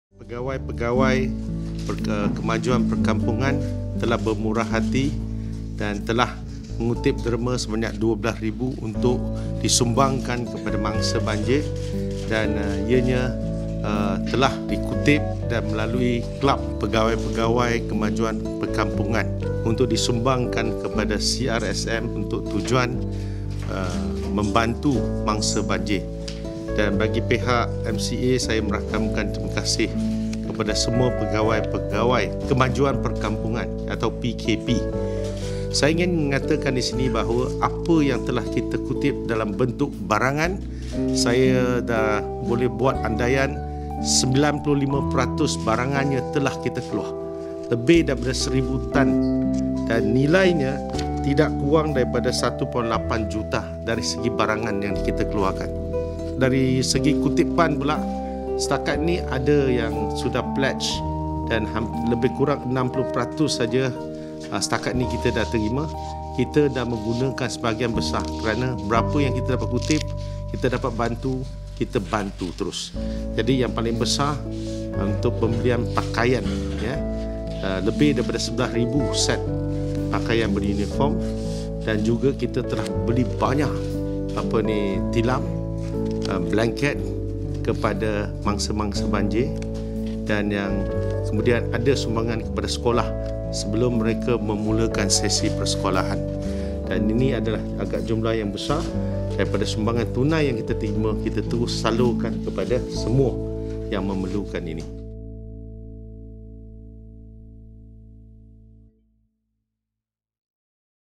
Datuk Wee berterima kasih kepada semua penderma & CRSM
Datuk Wee berterima kasih kepada semua penderma dan CRSM yang membantu dalam masalah banjir(MCA OFFICIAL)